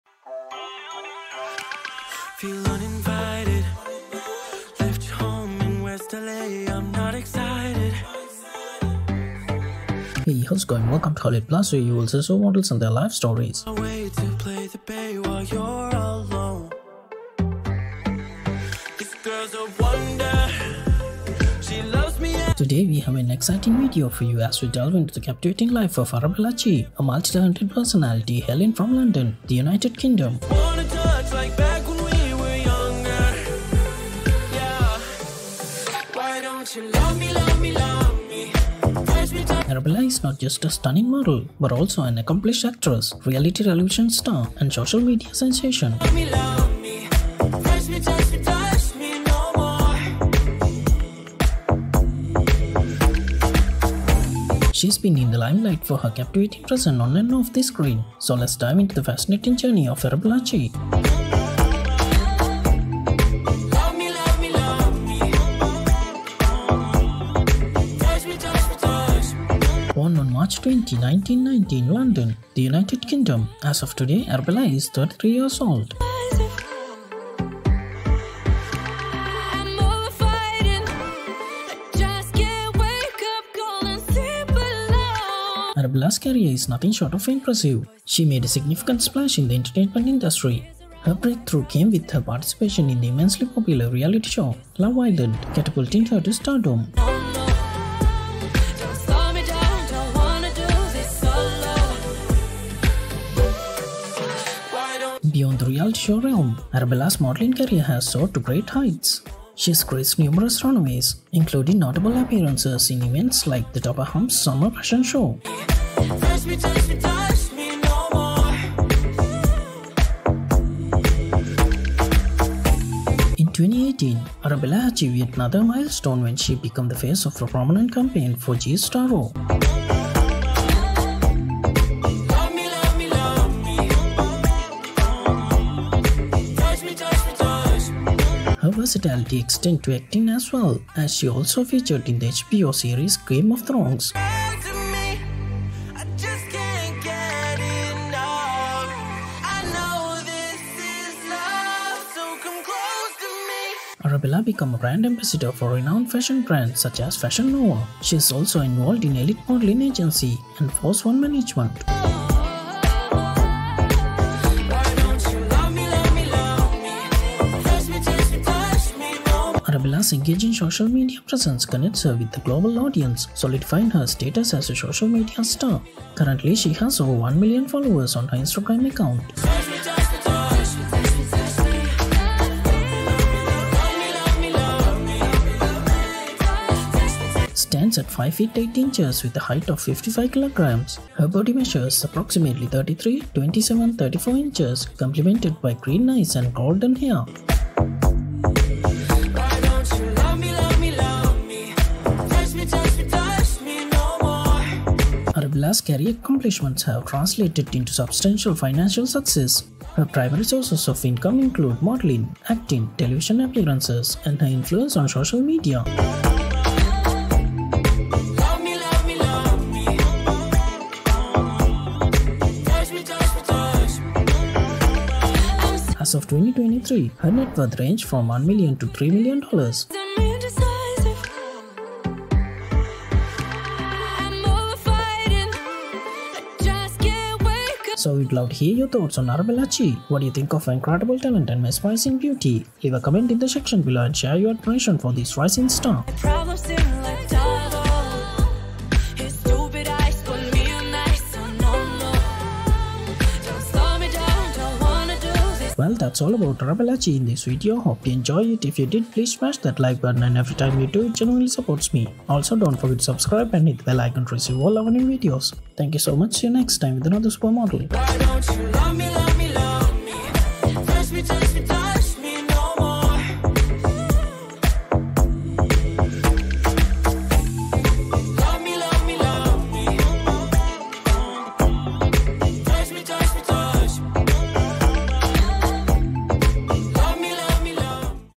Arabella Chi: British Model & Influencer | Biography & Insights
Today, we have an exciting video for you as we delve into the captivating life of Arabella Chi, a multi-talented personality hailing from London, the United Kingdom. born on March 20, 1990. Arabella's career is nothing short of impressive. She made a significant splash in the entertainment industry. Her breakthrough came with her participation in the immensely popular reality show, Love Island, catapulting her to stardom. Arabella Chi stands at 5 feet 8 inches with a weight of 55 kg. Her figure measures approximately 33-27-34 inches complemented by green eyes and golden hair. So, we would love to hear your thoughts on Arabella Chi. What do you think of her incredible talent and mesmerizing beauty?

* FYI*
There is never a need to strike a video down when you can get it removed within the same day and keep the channel and yourself happy!